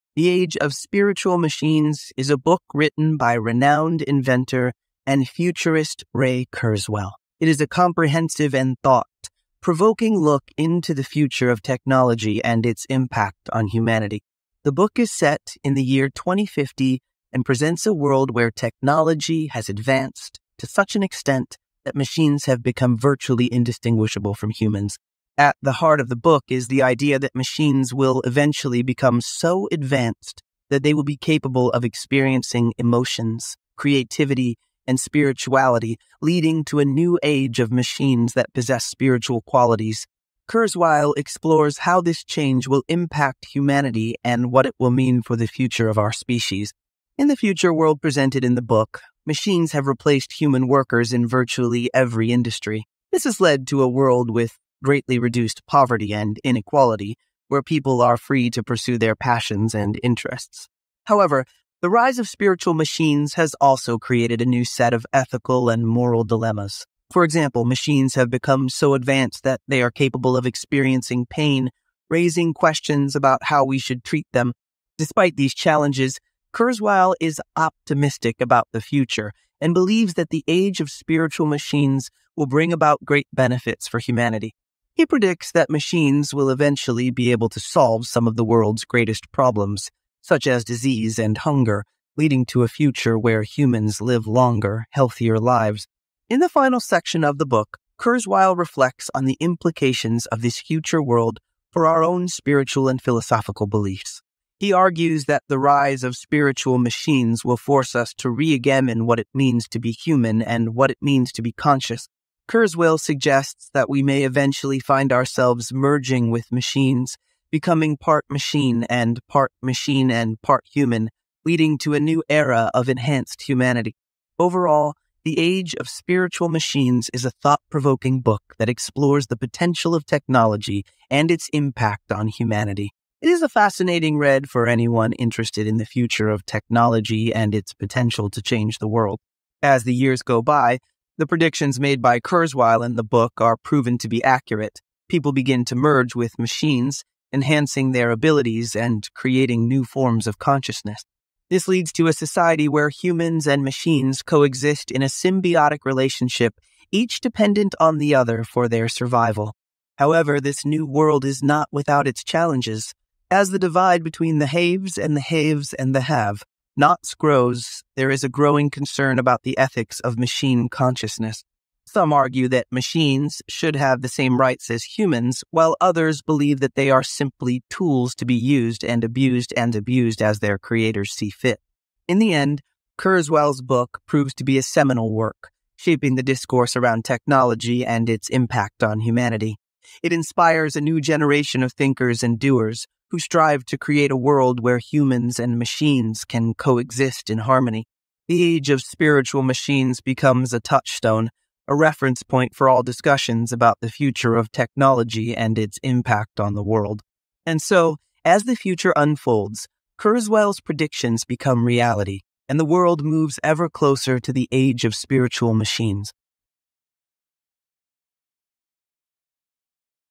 "The Age of Spiritual Machines" by Ray Kurzweil - Entire book in 4 minutes
Step into the future with "The Age of Spiritual Machines" by renowned inventor and futurist Ray Kurzweil. In this thought-provoking book, Kurzweil explores the potential impact of artificial intelligence on society and the progression of technology. Get a glimpse into the future of AI and its role in transforming our world. Watch now to expand your understanding of this rapidly evolving field.